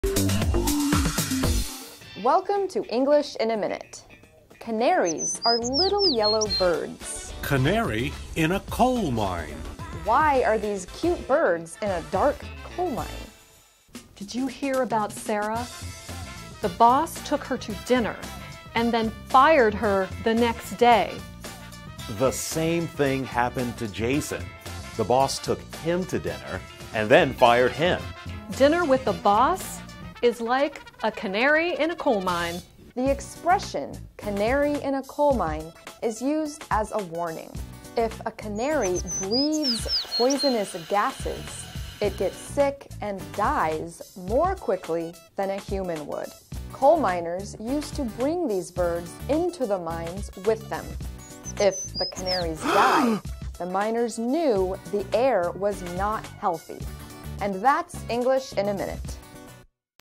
English in a Minute: Canary in a Coal Mine
What do small yellow birds have to do with a dark coal mine? Learn the meaning and history of this expression with Anna, Jonathan and Ashley.